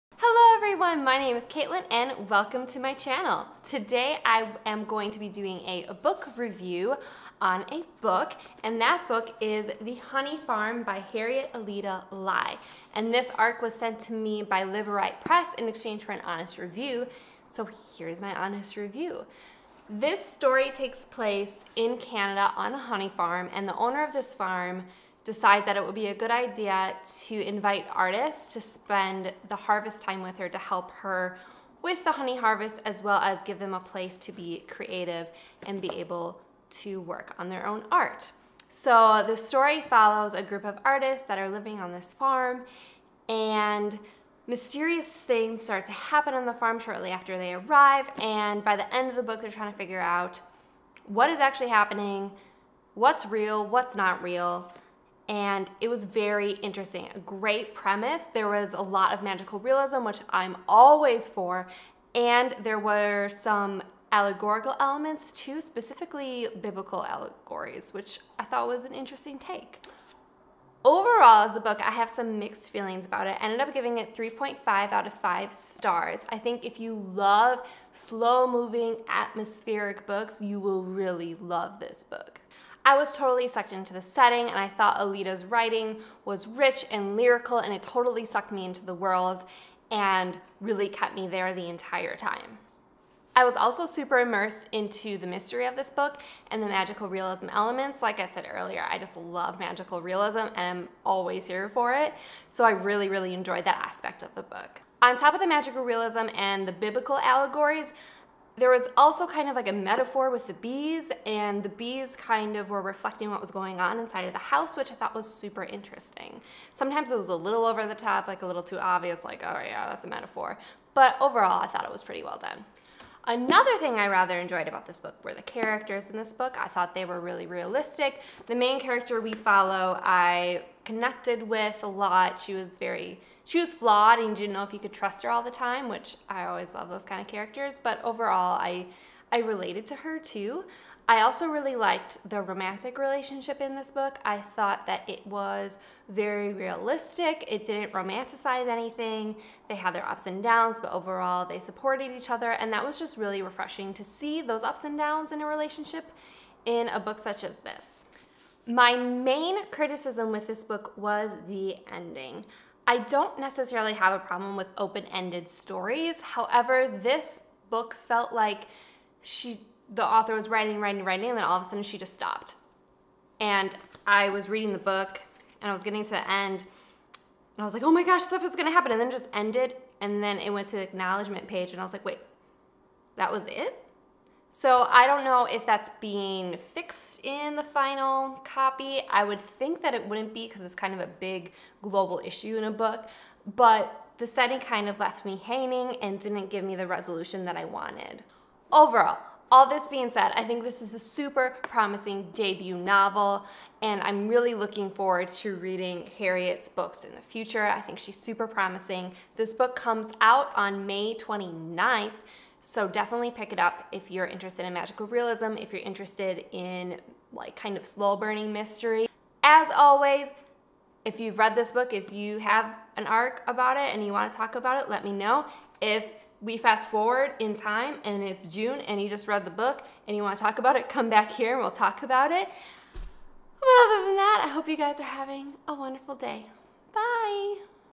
The Honey Farm by Harriet Alida Lye-BOOK REVIEW
Thank you Liveright and W.W. Norton & Company for sending this book.
The Honey Farm will be in bookstores on May 29!